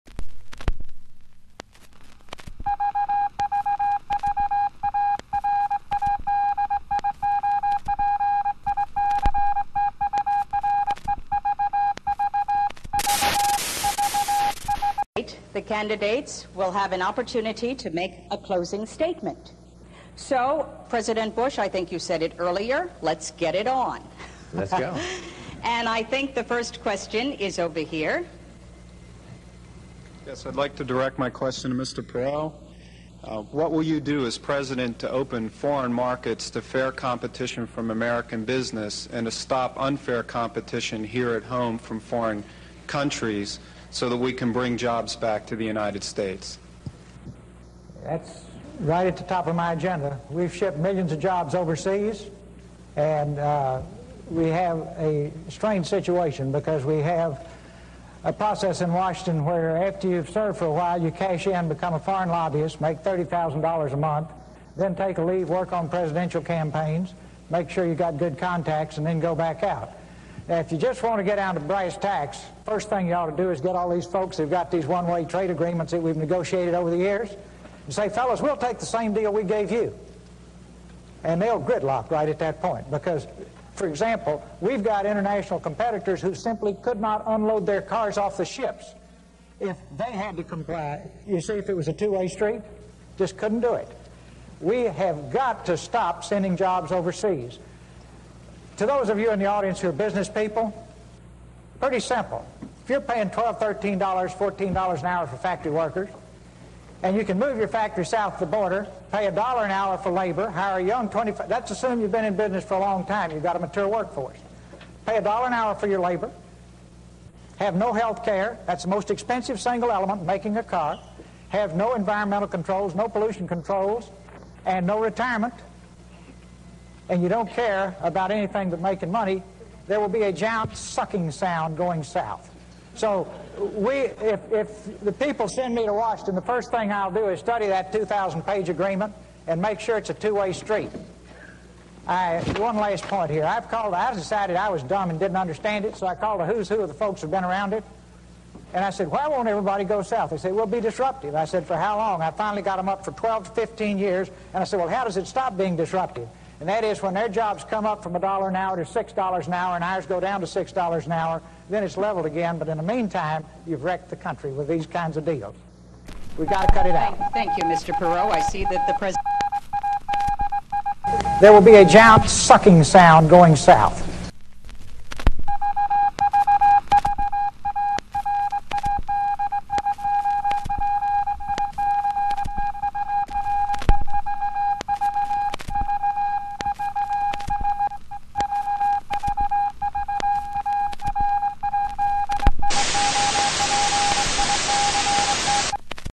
Ross Perot Presidential Debate 1992 - Giant Sucking Sound
For those who don't remember or weren't around yet, I present Ross Perot. Listen to what Ross Perot had to say about so-called "Free Trade" agreements like NAFTA. Let it digest a little bit and think to yourself, "Was Ross Perot right?"

This was back in 1992. Ask yourself, do you feel economically more secure now than 1992? Do you feel we have a bright economic future ahead?

When certain candidates in 2016 talk about so-called free trade deals being bad maybe it is because they actually are bad. Another candidate thinks these deals are great and wants to keep them.

Figured I would throw that in there.

for more current events, memes, and videos